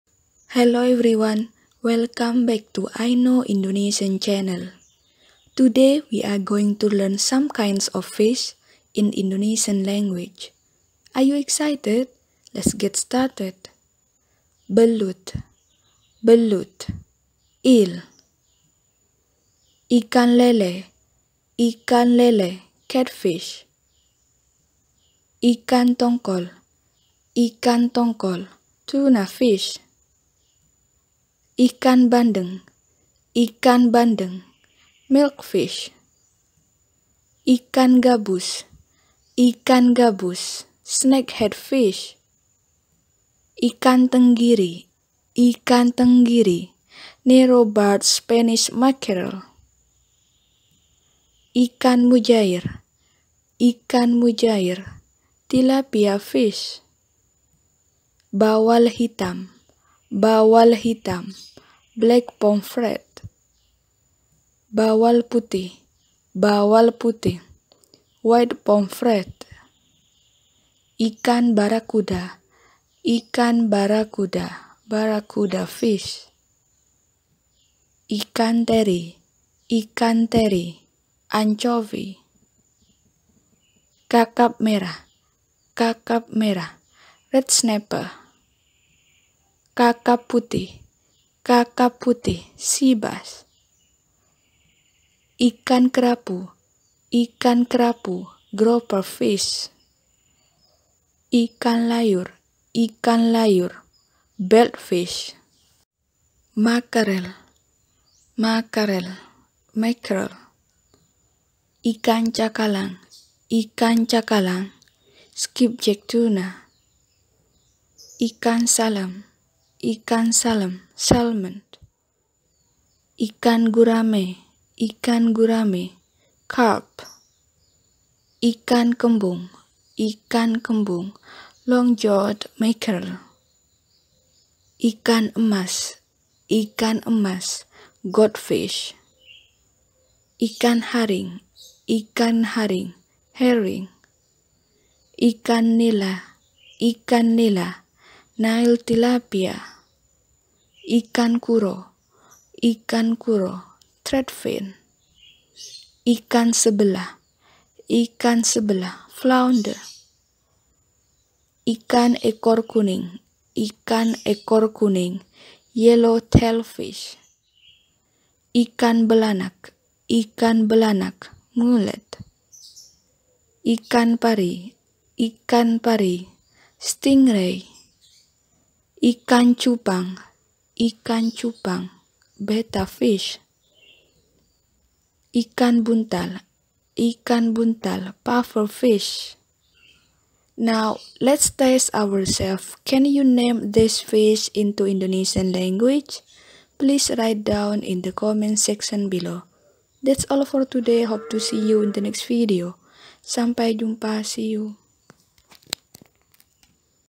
[VOCABULARY] Learn Some Kinds of Fish in Indonesian Language
This video is about some kinds of fish in Indonesian language.